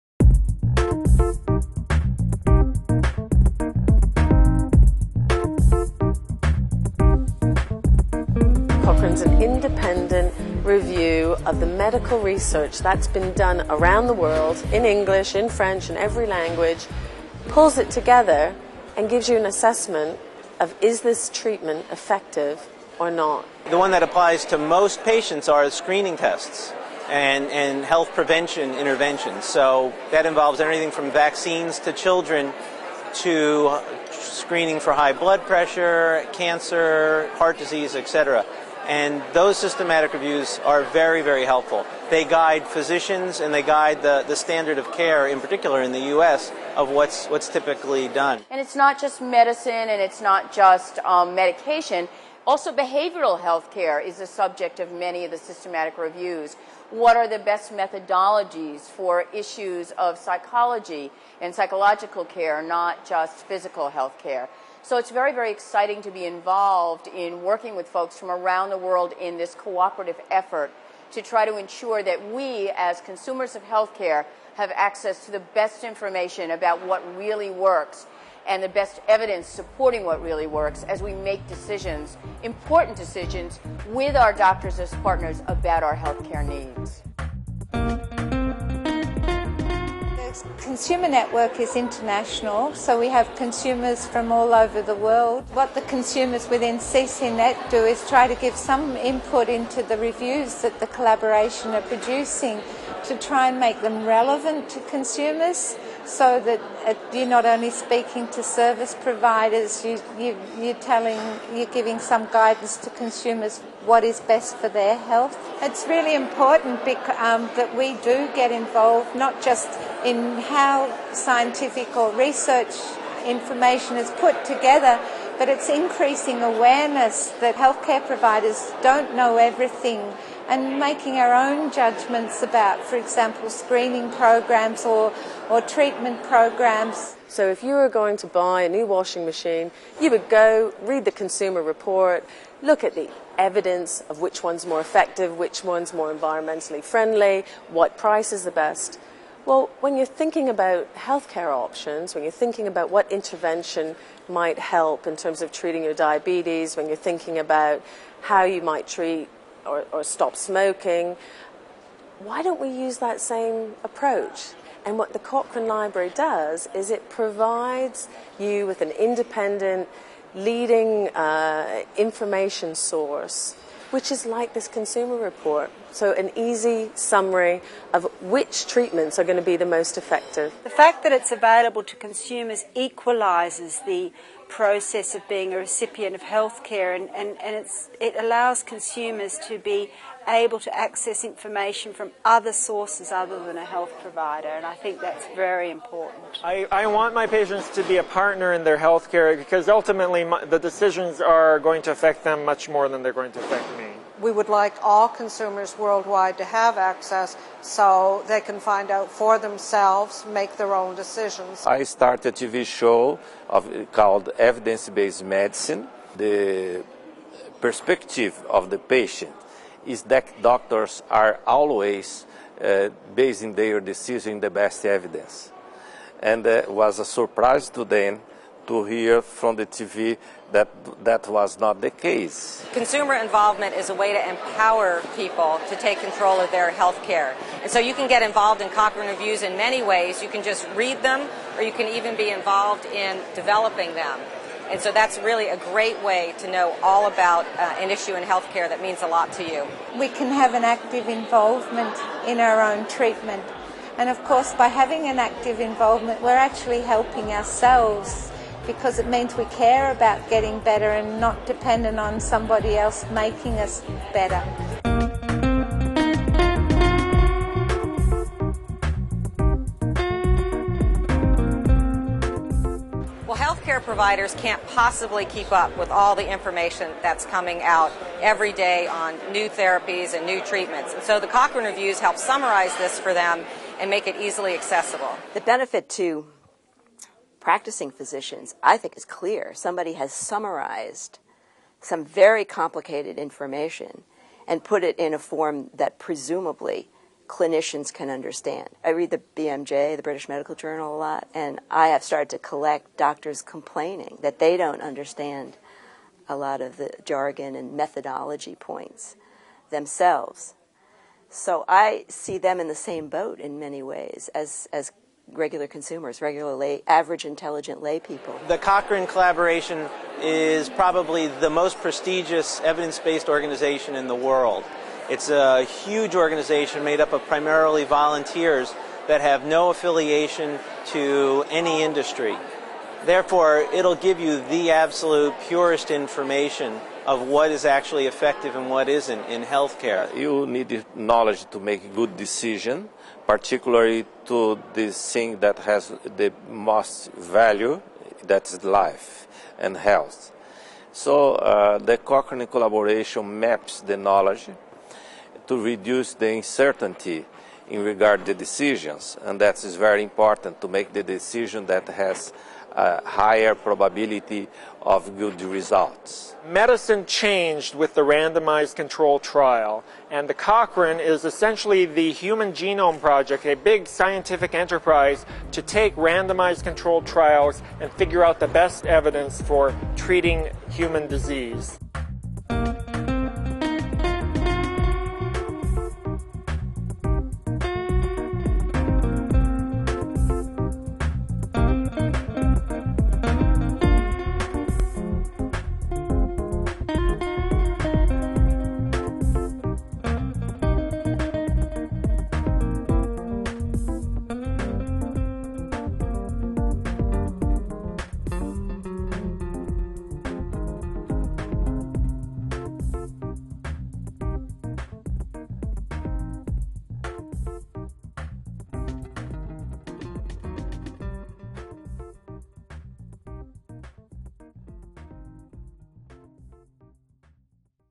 What is the Cochrane Collaboration?
This 7-minute Video FAQ explains the workings of the Cochrane Collaboration and the Cochrane library and how these benefit both consumers and providers. The Cochrane Collaboration is an international not-for-profit and independent organization, dedicated to making up-to-date, accurate information about the effects of healthcare readily available worldwide. It produces and disseminates systematic reviews of healthcare interventions and promotes the search for evidence in the form of clinical trials and other studies of interventions. The Cochrane Collaboration was founded in 1993 and named after the British epidemiologist, Archie Cochrane. The major product of the Collaboration is the Cochrane Database of Systematic Reviews which is published quarterly as part of The Cochrane Library. Those who prepare the reviews are mostly healthcare professionals who volunteer to work in one of the many Cochrane Review Groups, with editorial teams overseeing the preparation and maintenance of the reviews, as well as application of the rigorous quality standards for which Cochrane Reviews have become known. The activities of the Collaboration are directed by an elected Steering Group and are supported by staff in Cochrane Entities (Centres, Review Groups, Methods Groups, Fields/Networks) around the world.